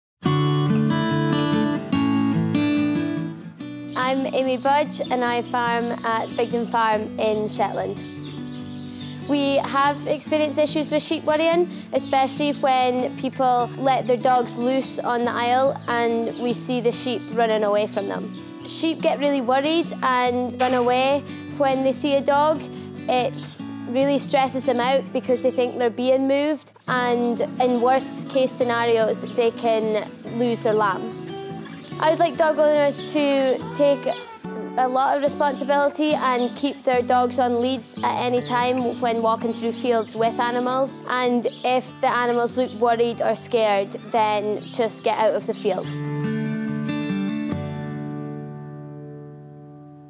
Lambing season advice for Shetland dog walkers
For more useful info on access rights during the coronavirus outbreak
Follow ShetlandOutdoors on Twitter